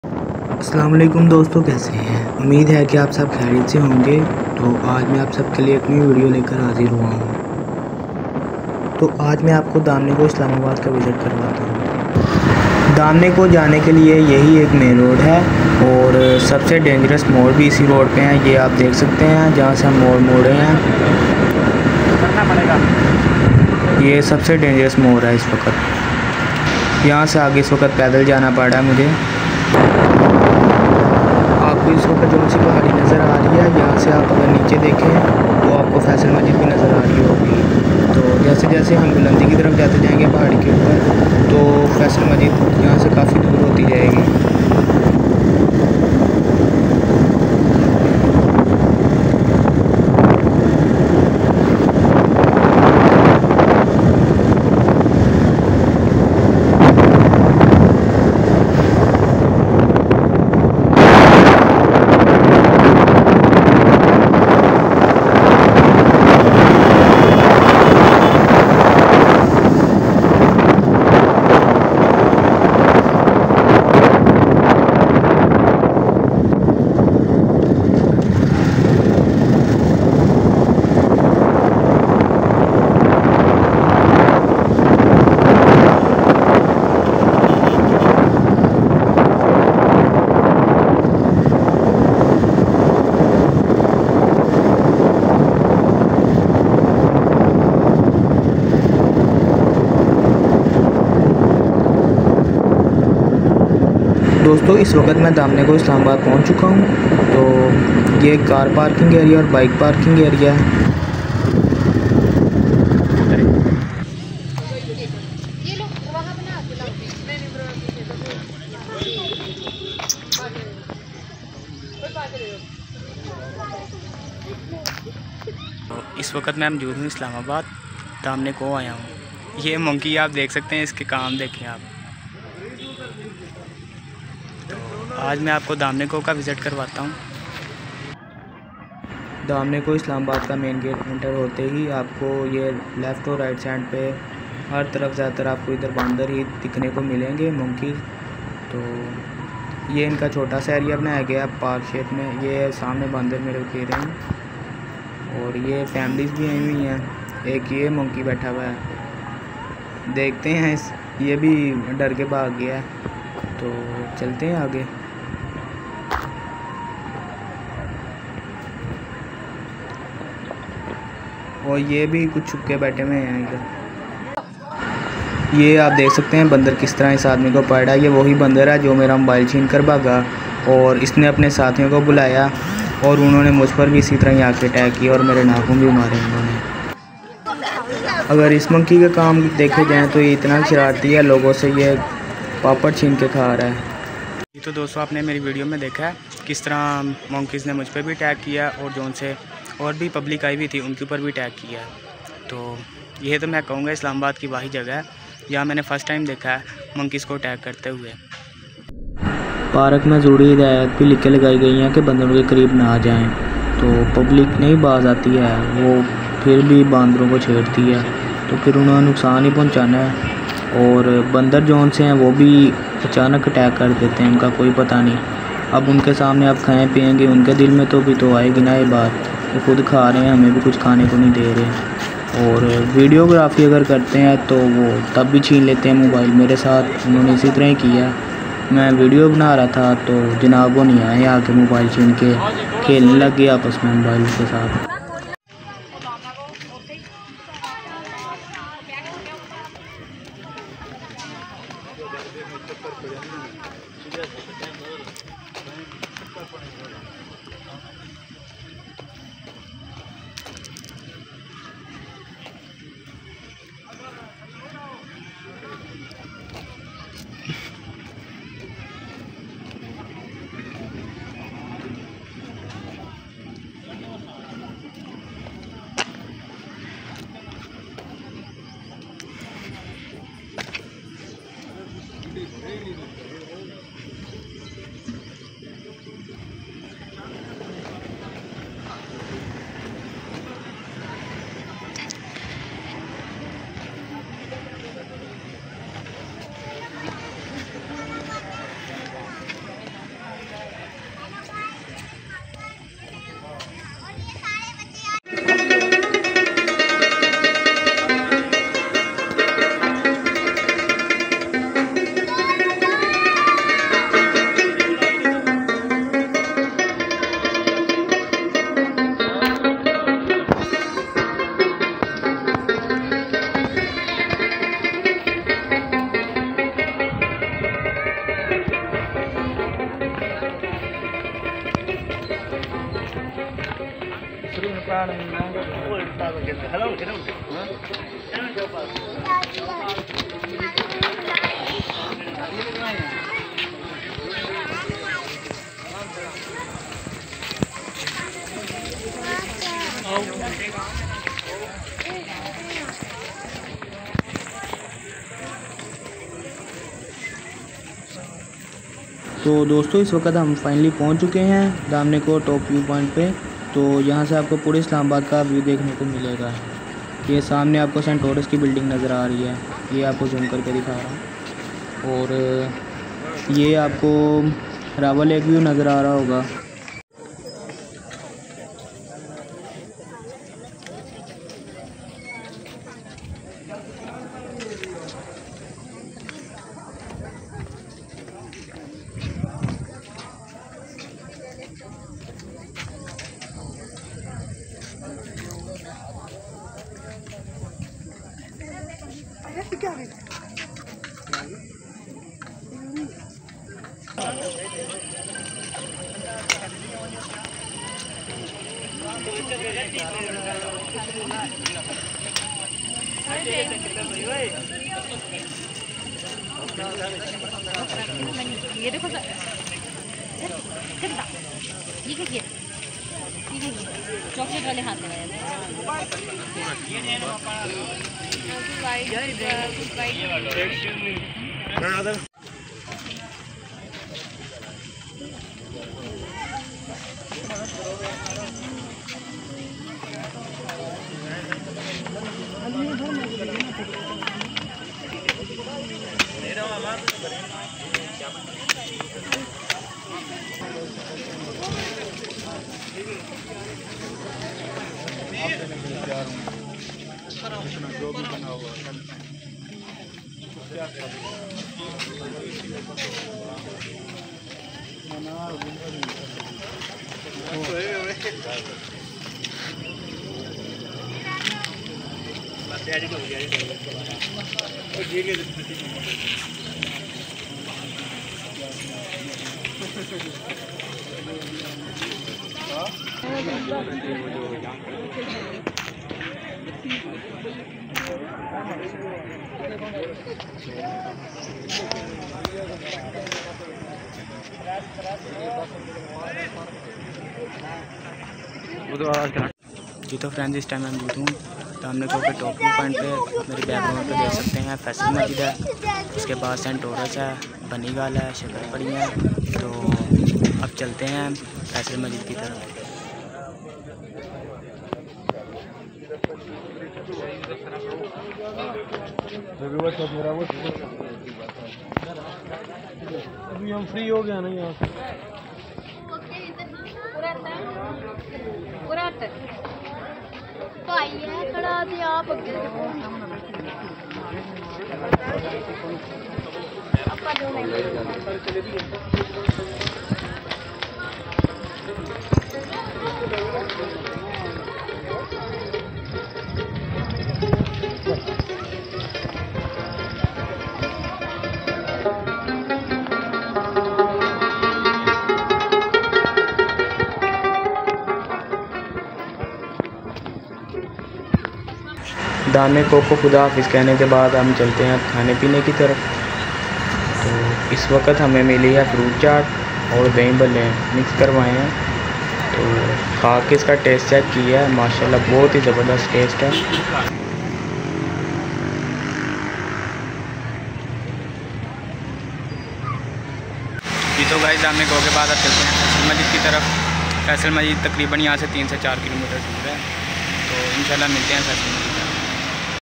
Damn e koh Islamabad || Margalla hills || Monkeys of damne koh Islamabad 😱
You must have seen in my old vlog that I traveled by bus to Islamabad and today I have come to give you a tour of the foothills of Islamabad.
Daman-i Koh Margalla Hills is a picnic point in Islamabad from where you will get a view of the entire Islamabad. Mischievous monkeys are also seen harassing people here. Friends, these monkeys also did a lot of harm to me, snatched my mobile phone and ran away and even injured me by killing my nails. I request all of you to protect yourself and your belongings at this point.

👉 Host : M.Hamza ijaz from (Faisalabad).